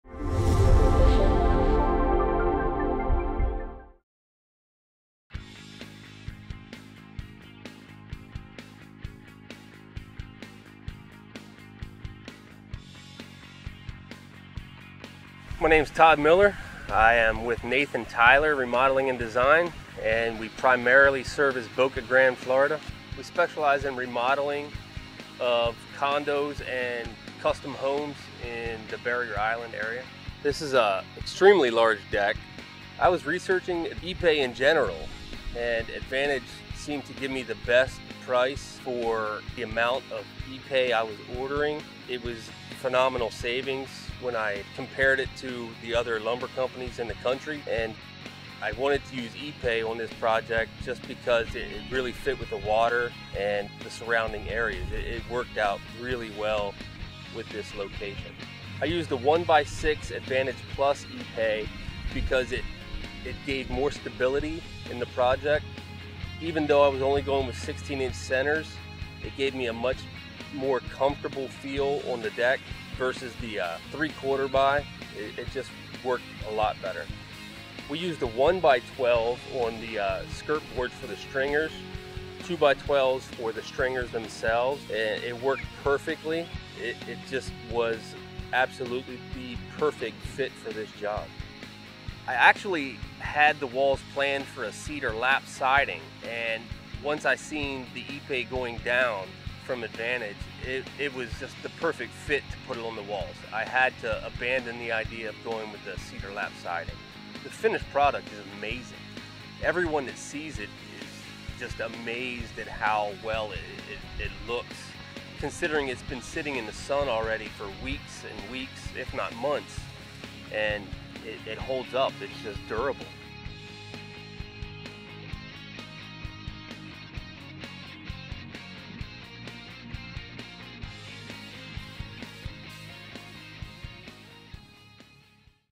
Advantage Ipe™ Deck Project in Boca Grande, FL
What sets 1x6 +Plus Ipe apart from thinner decking material? Discover what Todd Miller of Nathan-Tylor Remodeling in Boca Grande, FL was able to build using 1x6 +Plus Advantage Ipe decking. From the deck, to stairs, to siding, this massive deck serves as the ultimate outdoor living space.

Hardwood decking, deck tiles, live edge slabs, and more. We ship directly to your home or job site worldwide.